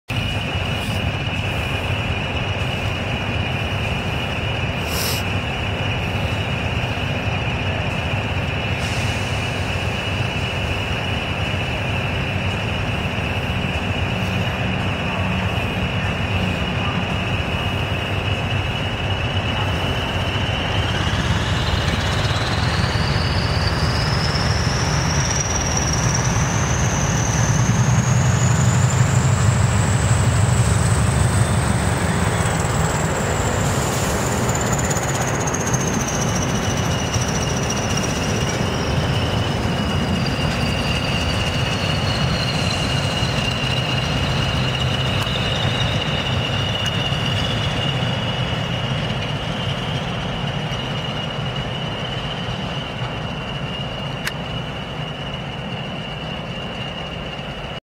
20305/302/303 & 312 depart Doncaster
working light engine from Carriage Holding Sidings to Doncaster Up Decoy in preparation for an engineers train, Enjoy :)